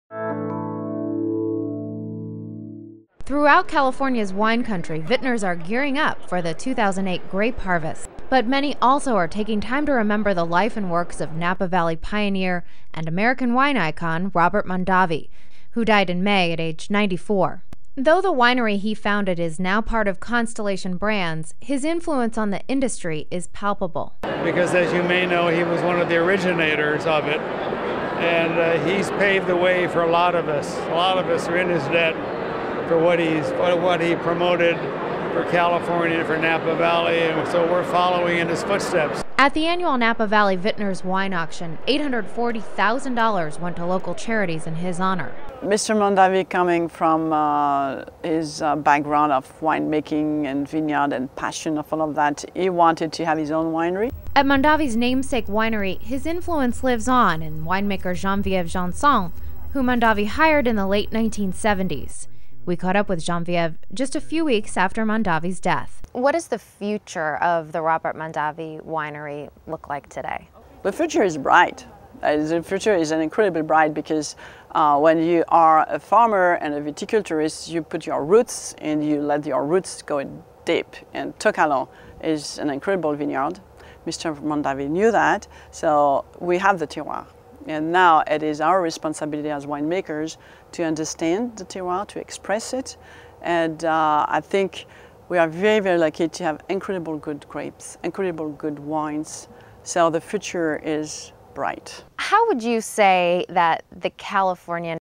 Life and Works of Robert Mondavi
As Napa vitners prepare for this fall's grape harvest, many are taking time to reflect on the life and influence of Robert Mondavi on the wine industry. Stacey Delo talks with Genvieve Janssenns, the wine maker for the Robert Mondavi Winery. (July 3)